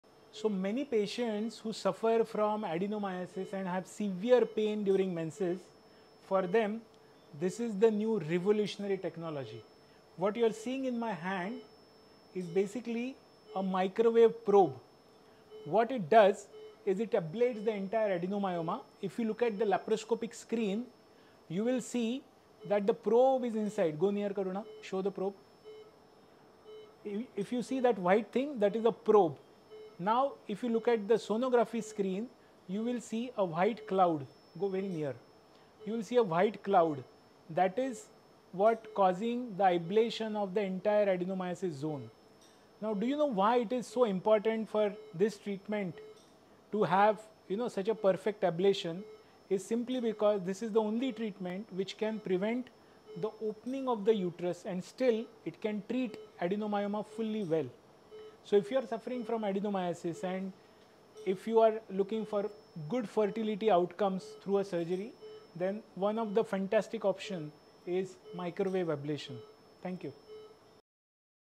New hope for adenomyosis patients!
Adenomyosis patients often struggle with severe period pain and face difficulty conceiving.
This new microwave ablation technology offers a breakthrough solution.

With the help of a microwave probe, the adenomyoma is completely ablated without opening the uterus.
This means faster recovery, less pain, and better fertility outcomes.

If you are suffering from adenomyosis and looking for an effective treatment that preserves fertility,
microwave ablation can be a powerful option.

ADDRESS - ISHA WOMEN’S HOSPITAL, DOMBIVLI EAST, MAHARASHTRA, INDIA. FOR APPOINTMENT CALL - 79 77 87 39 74